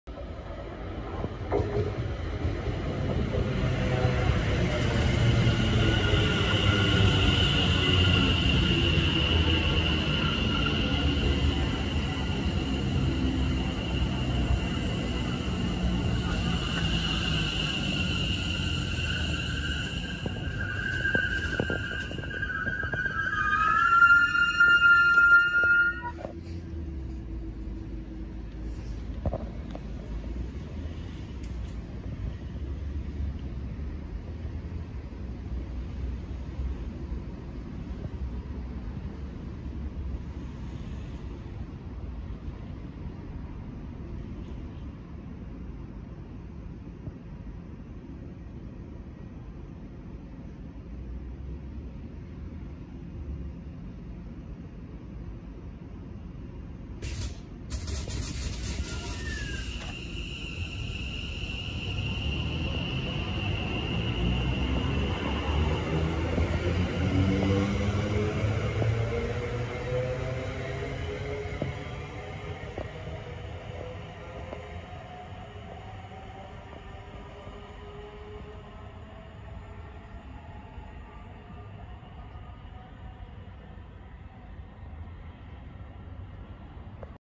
Greater Anglia Class 720 (720521) Arriving and Departing Brimsdown for London Liverpool Street
British Rail Class 720
720121
Greater Anglia
From Hertford East to London Liverpool Street via Tottenham Hale
Formed of 5coaches